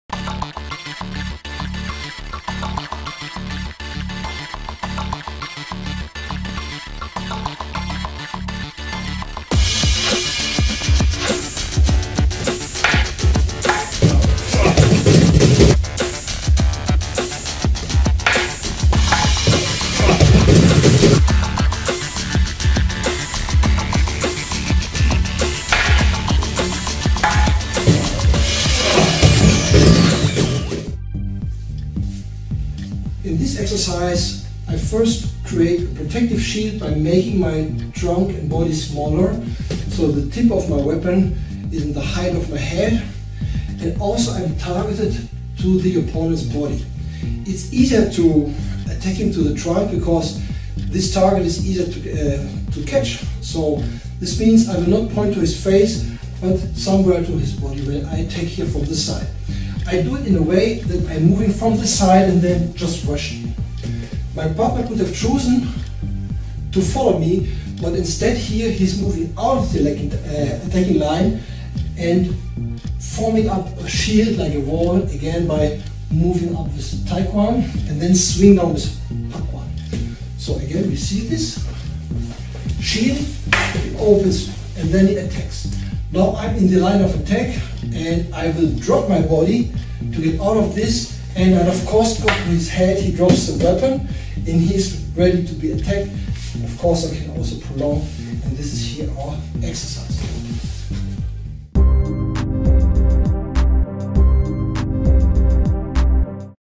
DRAGOS WING TSUN Longpole EducAction Shortclip No 9
Lok Dim Boon Kwun Applications - Sifu Martin Dragos

I first move closer to  opponent by hiding behind a shield. He moves out of attacking line and swings up by TaiKwan to protect, then creates an opening by downward strike with PakKwan..as my pole was in BiuKwan and extended, this lets my pole tip drop down..because my partner now attacks my upper trunk area I decide to quickly sink to my knees by GwaiMa and will disarm the attacker to its wrist by HuenTaiKwan..then I strike with my counterattack...Enjoy and visit dragoswingtsun.com ps. Hope you are not disappinted we do not really hurt each other during that demo..safety first;-)

Greetings Sifu Martin